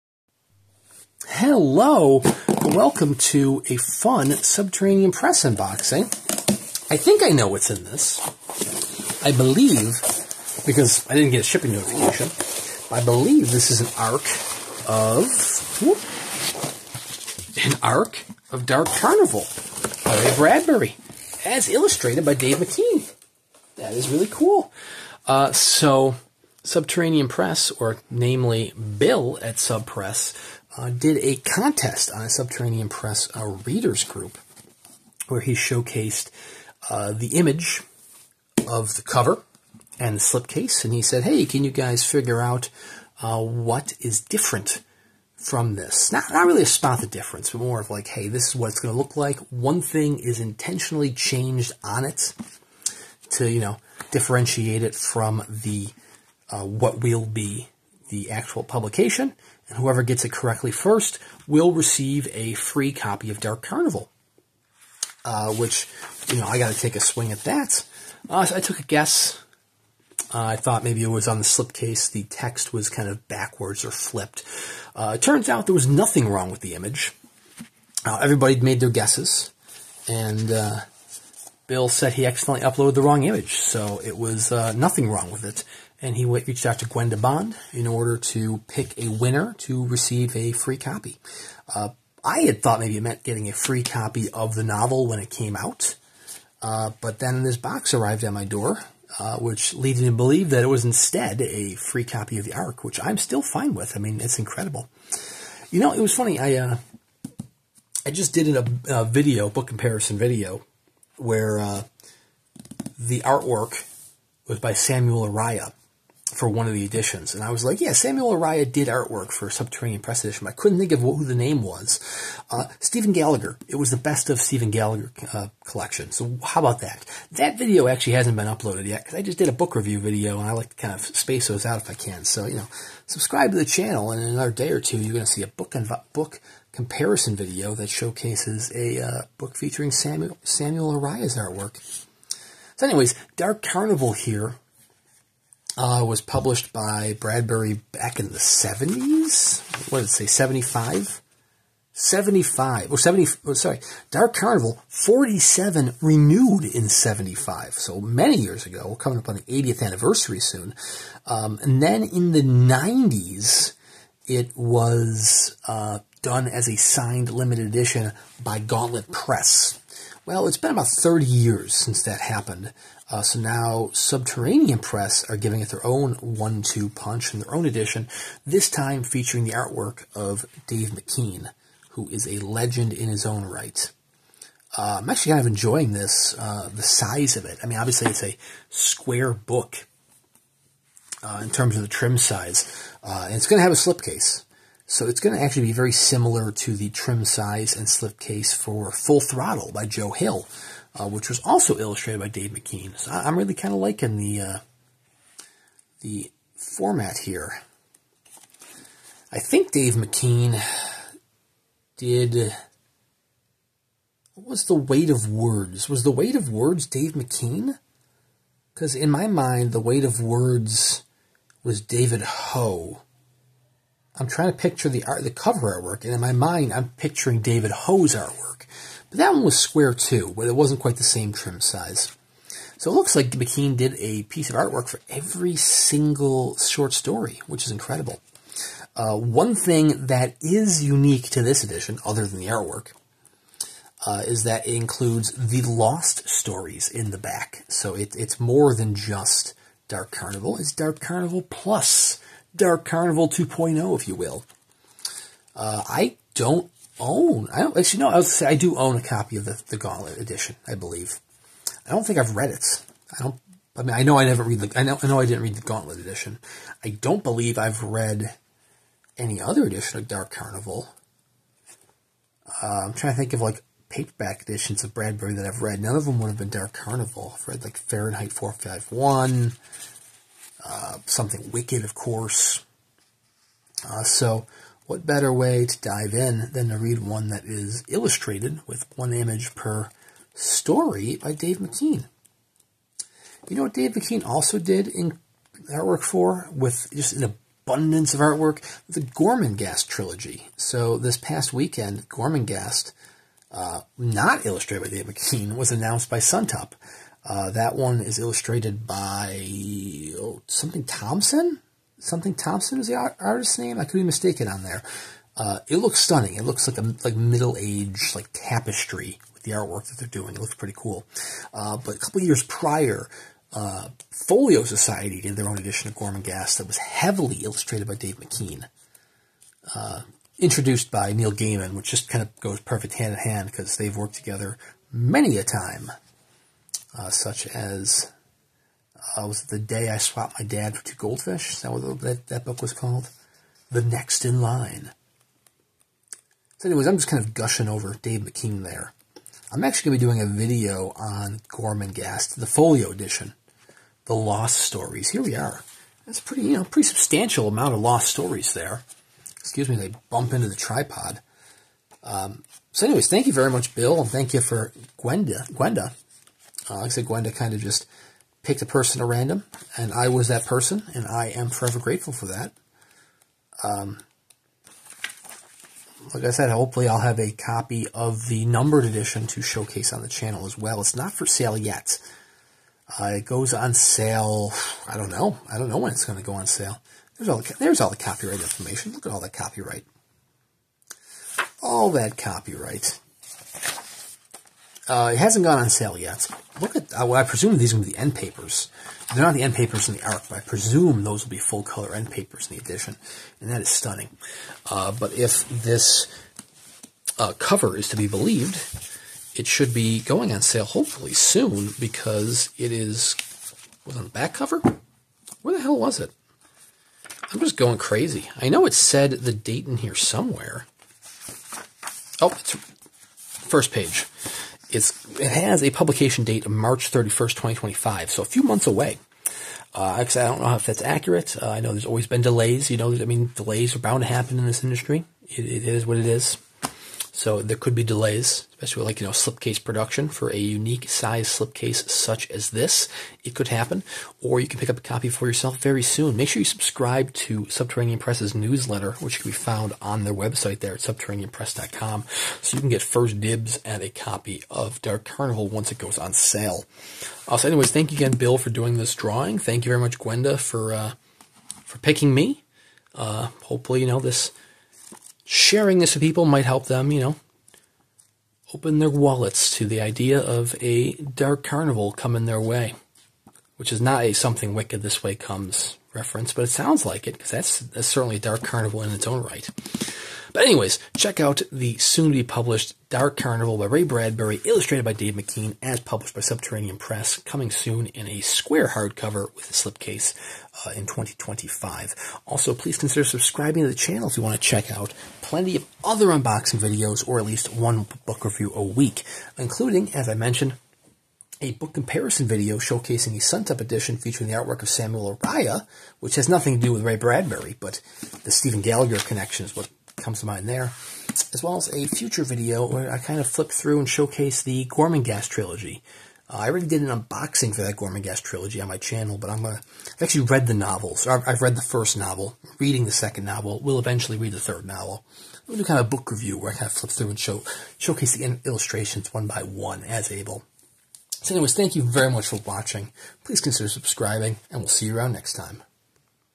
Unboxing an ARC for Dark Carnival by Ray Bradbury - Illustrated by Dave McKean - Subterranean Press
Unboxing an ARC for Dark Carnival by Ray Bradbury, soon to be published by Subterranean Press. This edition will be slipcased and heavily illustrated by Dave McKean.

Thank you very much to Gwenda and Bill for this ARC!